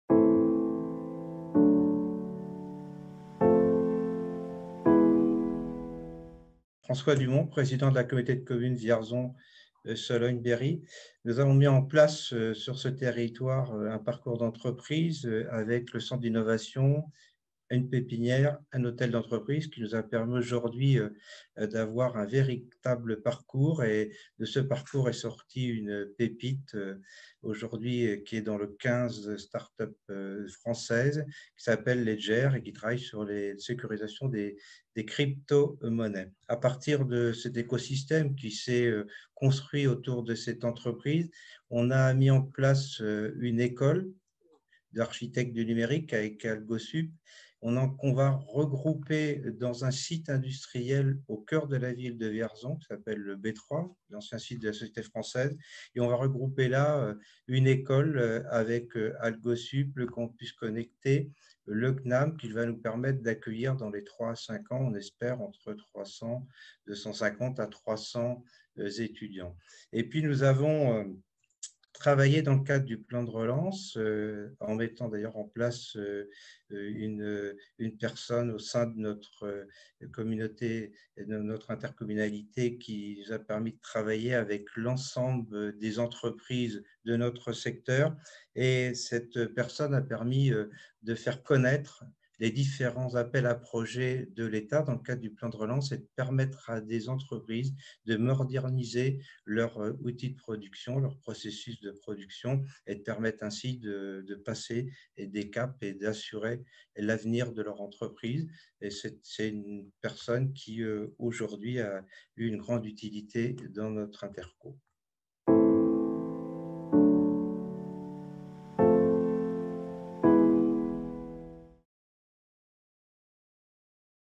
Au cœur de la relance avec François Dumon, président de Vierzon-Sologne-Berry
« Nous allons également essayer d’attirer des personnes qui ne sont pas sur le territoire »

François Dumon est président de la communauté de communes Vierzon-Sologne-Berry. Pour la série « Au cœur de la relance », il revient sur l’accompagnement personnalisé que l’intercommunalité offre à ses entreprises. Avant la crise sanitaire, elle était déjà très mobilisée pour donner toutes les clés de réussite aux acteurs du territoire. La crise sanitaire a amplifié cet élan, jusqu’à la création d’un poste d’ambassadrice des entreprises, dans le cadre de la relance. Un campus numérique va également être créé, et le parc technologique agrandi.